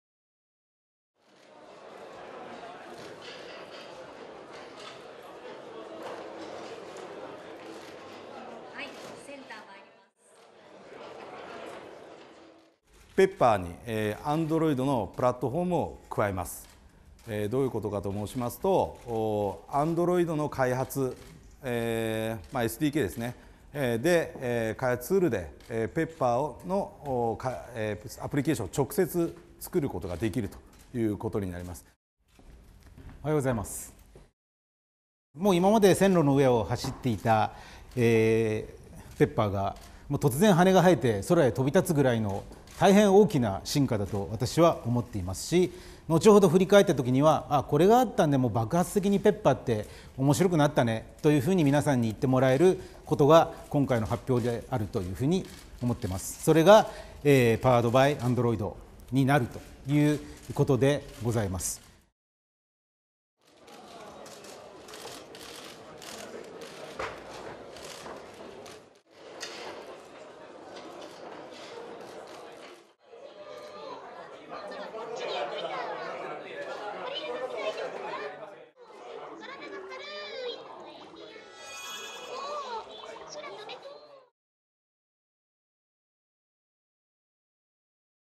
ペッパーの使いやすさ向上 米グーグルのＯＳに対応
ソフトバンクは１９日、人型ロボット「Ｐｅｐｐｅｒ（ペッパー）」を、米グーグルの基本ソフト（ＯＳ）「アンドロイド」に対応できるようにすると発表した。グーグルのアプリが使えるようになるなど使いやすさが大きく向上するという。